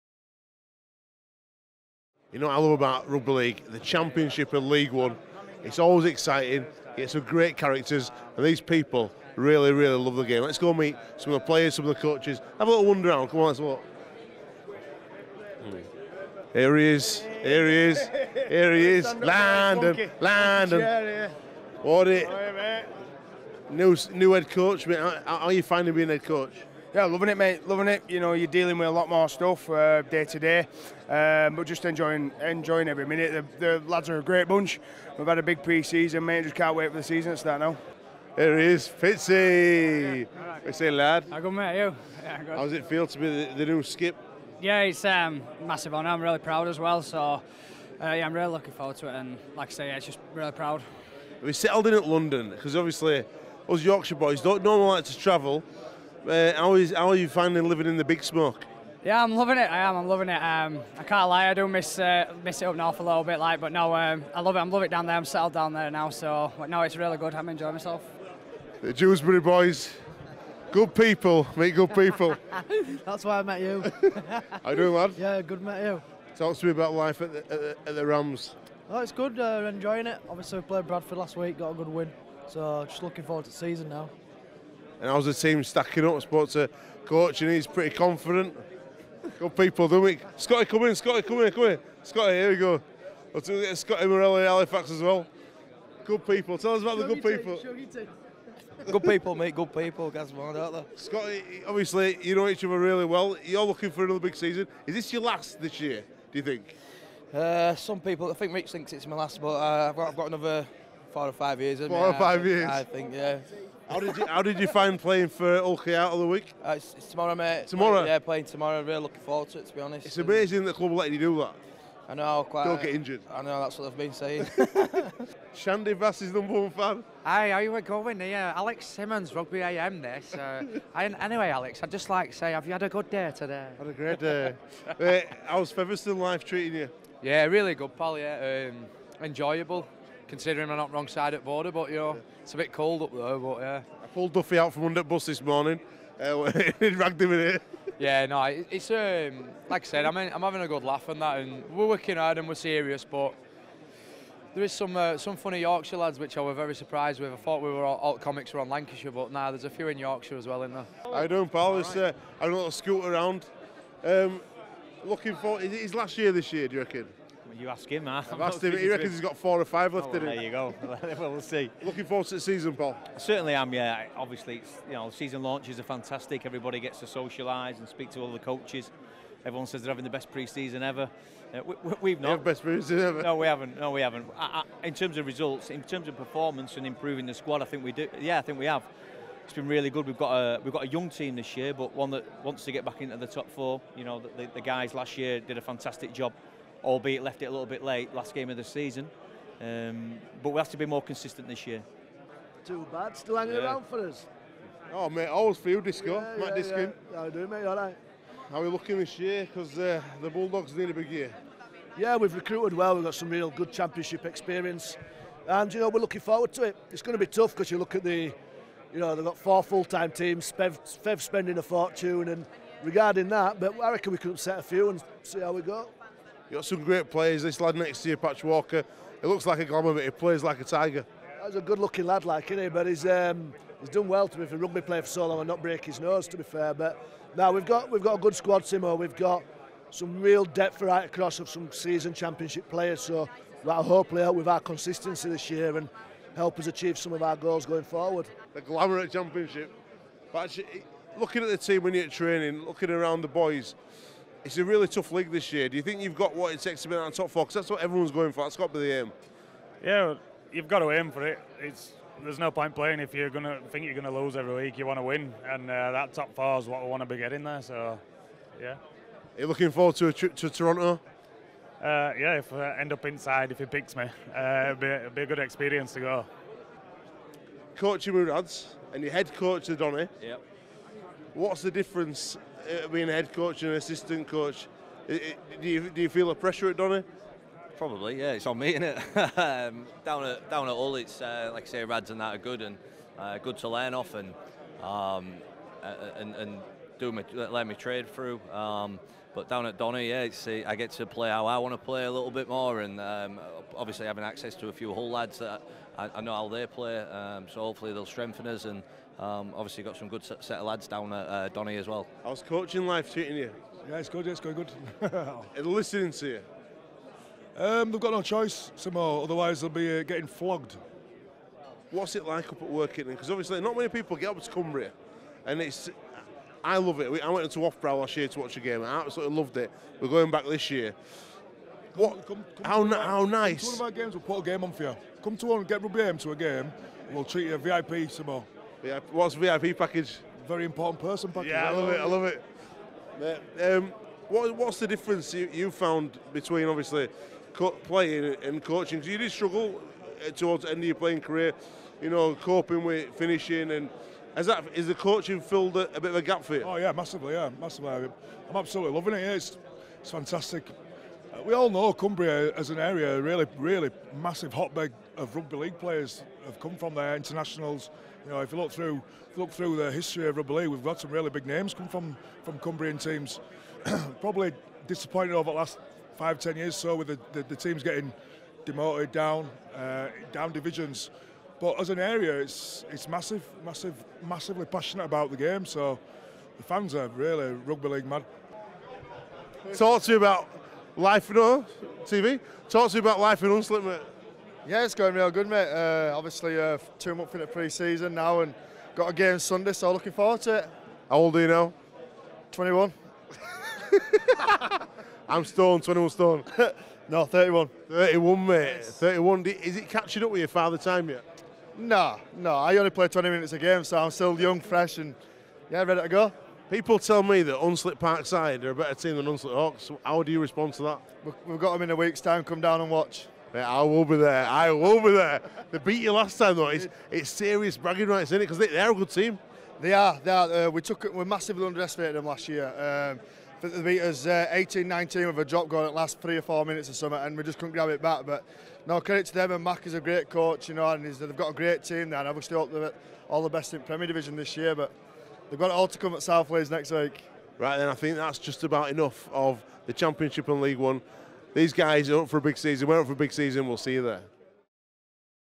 Championship and League 1 Launch
Alex Simmons interviews various Championship and League 1 players at the 2018 season launch event in Huddersfield.